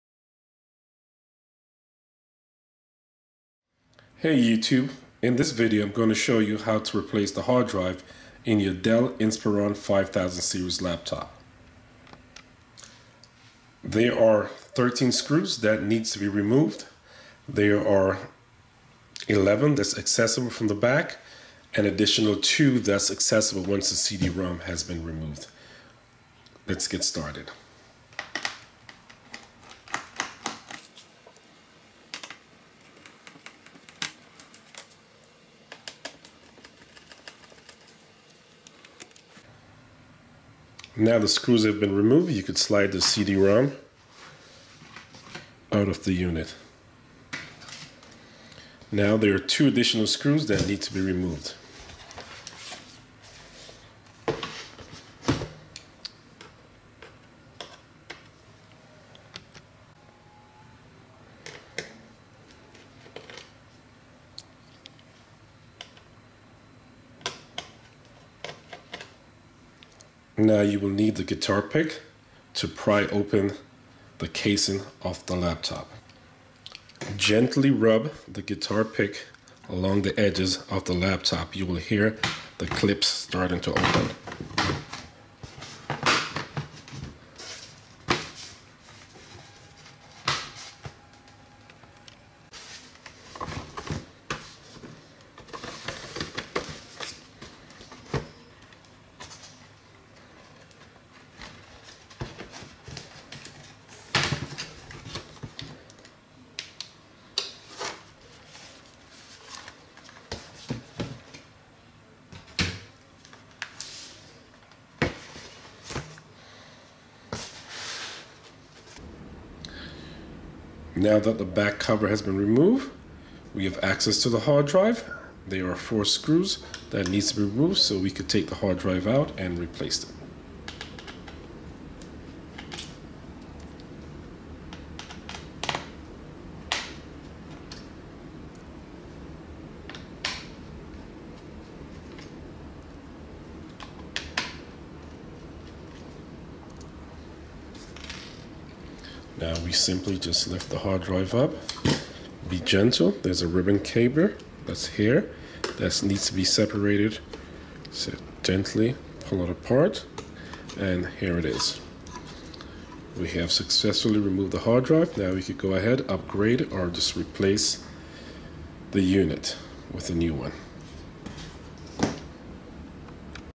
How to replace Dell Inspiron Hard drive
How to easily replace dell inspiron 15 5000 series hard drive.